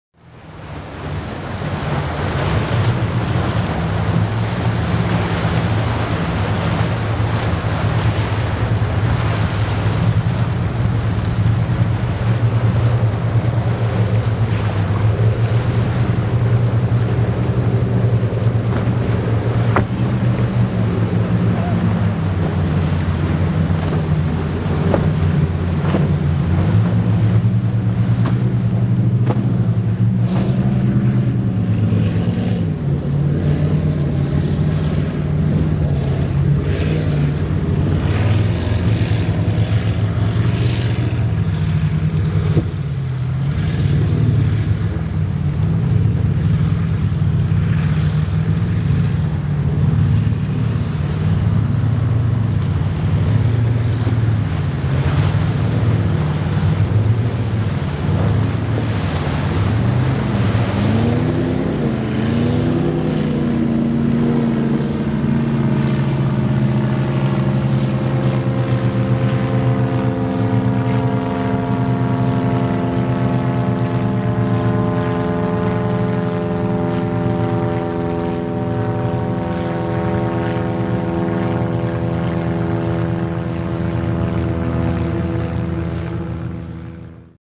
35 Super Sonic at Haulover Inlet
A bright 35 Super Sonic Powerboat exits Haulover Inlet and turns south on the Atlantic Ocean.